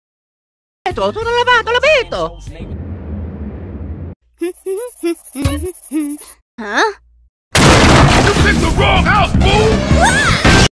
Meggy Abuse #3
Well, this time Mario triggered Big Smoke to attack and throw his whole body at Meggy. First was Mario throwing Meggy, Doc Ock attacking Meggy and now, Big Smoke throwing himself at Meggy.